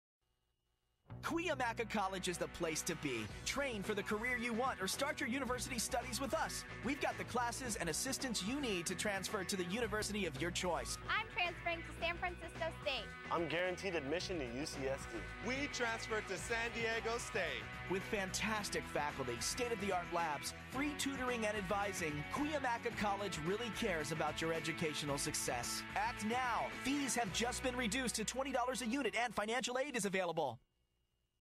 Cuyamaca College Transfer Video
Cuyamaca College is the place to be. Train for the career you want or start your University Studies with us. We've got the classes and the assistance you need to transfer to the University of your choice.